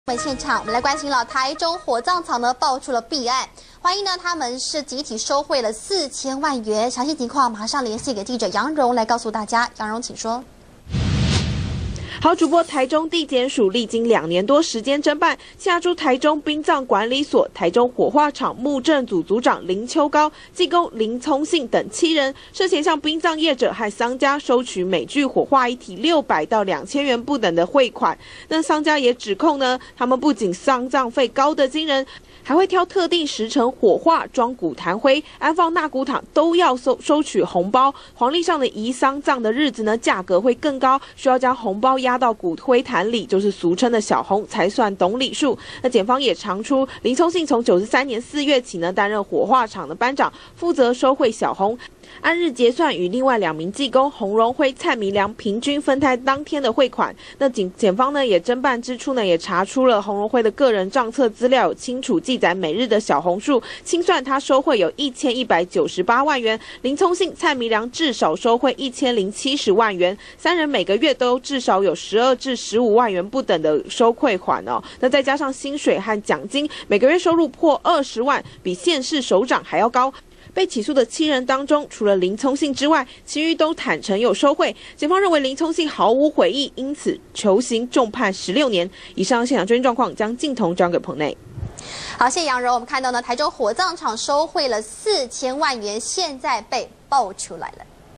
「沒錢死不起！」台中火葬場收賄4千萬
中天新聞》台中地檢署歷經2年多時間偵辦，清查出台中市殯葬管理所台中火化場墓政組組長林秋高、技工林聰信等7人，涉嫌向殯葬業者和喪家收取每具火化遺體6百至2千元不等的「小紅」，累計6年不法賄款近4千萬元，檢方偵結依貪汙罪嫌起訴；其中林聰信涉收賄上千萬又矢口否認，被求處16年重刑。